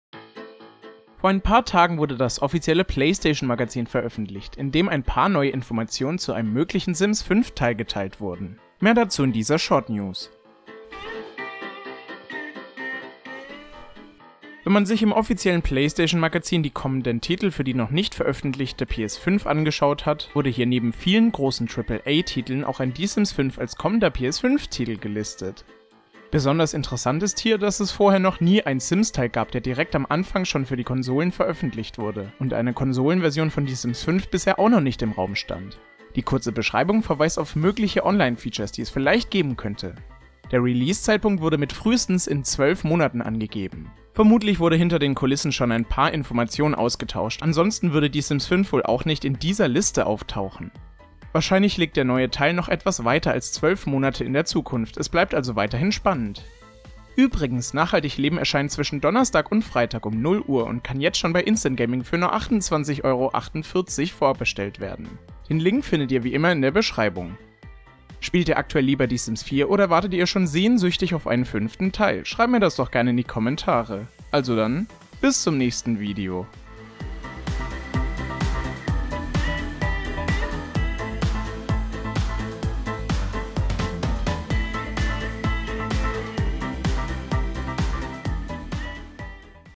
Die Sims 5 näher als gedacht & auch für PS5? | Short-News | sims-blog.de
In dieser Short-News möchte ich euch nur schnell auf einen Eintrag im offiziellen Play-Station-Magazin zu Die Sims 5 aufmerksam machen!